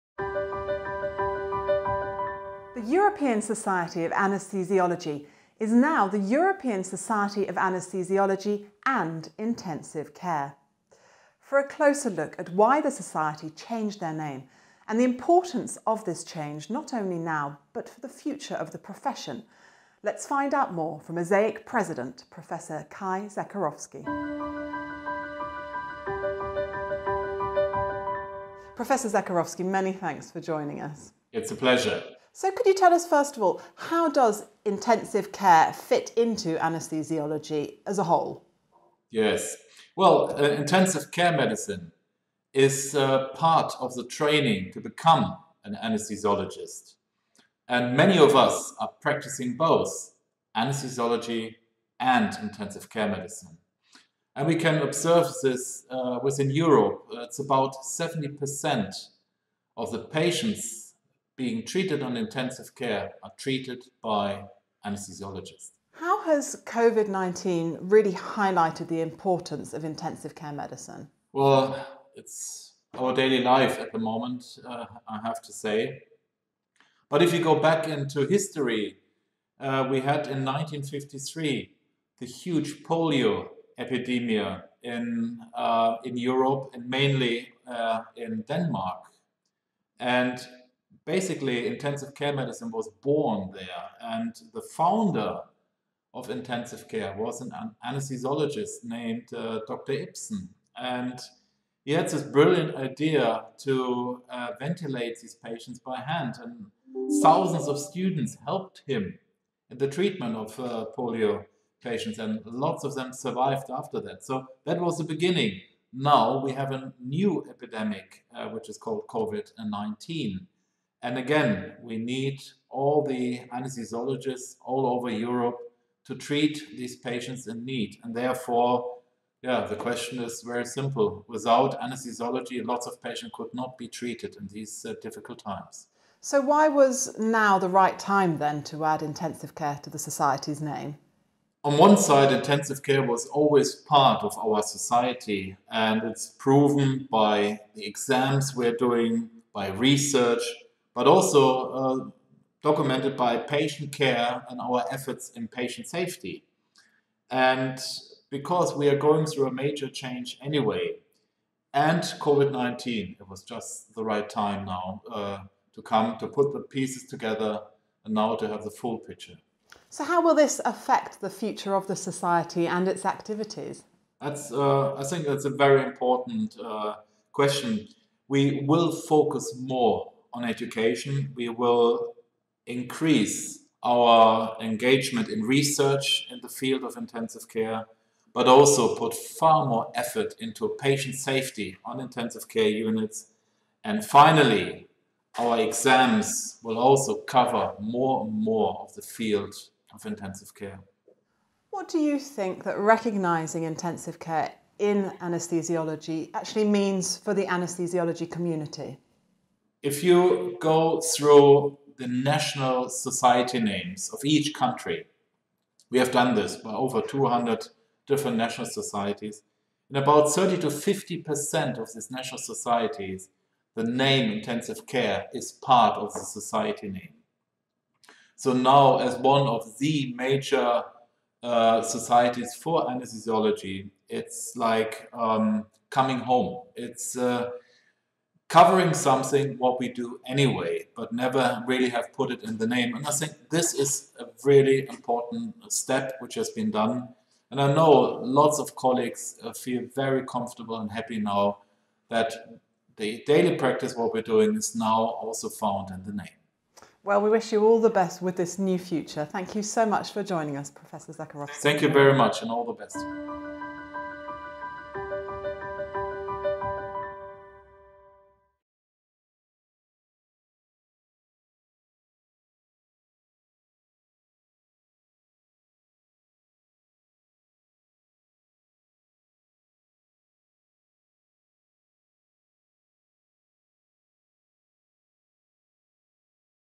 Intensive Care and the ESAIC
The European Society of Anaesthesiology is now the European Society of Anaesthesiology and Intensive Care. For a closer look at why the society changed their name, and the importance of this change not only now, but for the future of the profession, let’s have a conversation with ESCAIC President, Professor Kai Zacharowski.